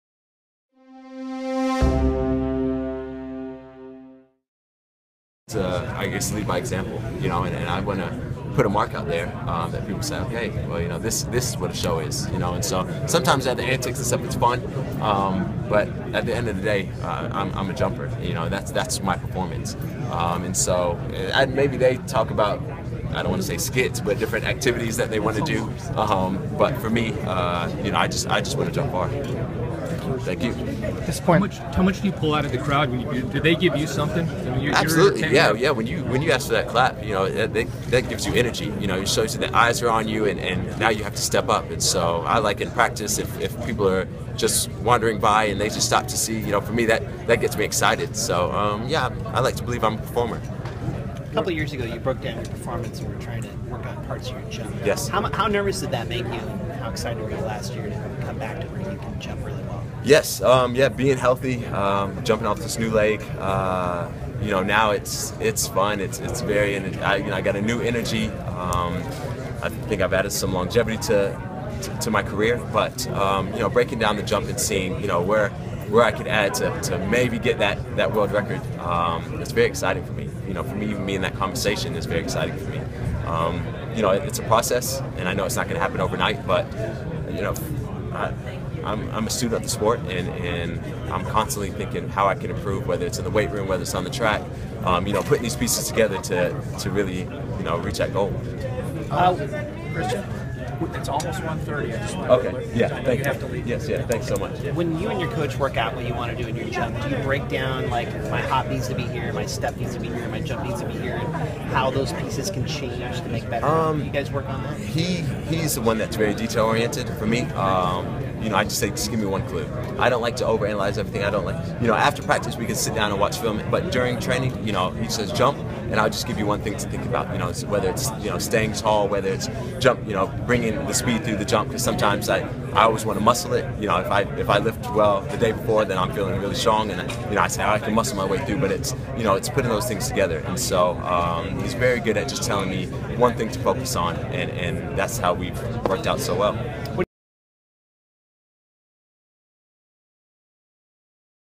Triple jumper Christian Taylor is a showman
Taylor, who will do his thing at the Pre Classic, says he draws energy from the Hayward fans.